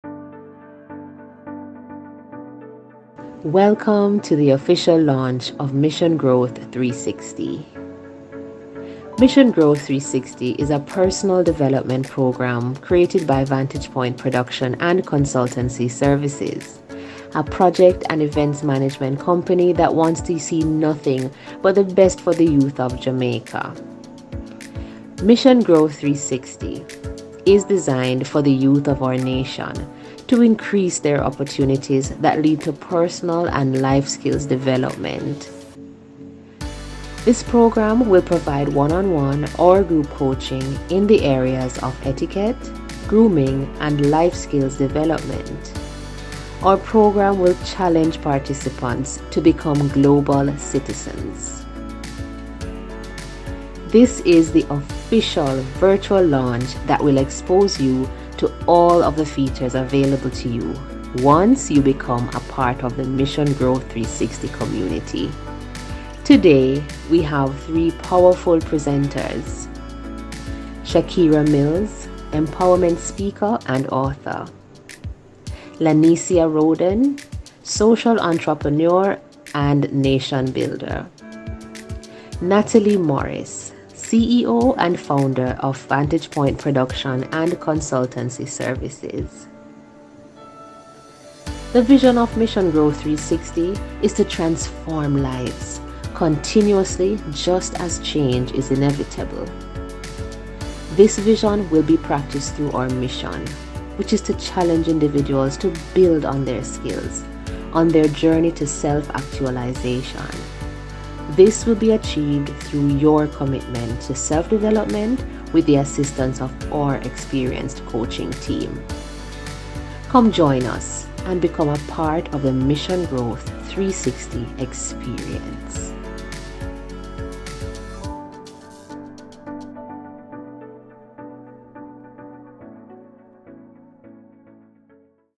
Personal Development Programme: Mission Growth 360 Overview
This is what you can expect when you join our personal development programme Mission Growth 360.

Telephone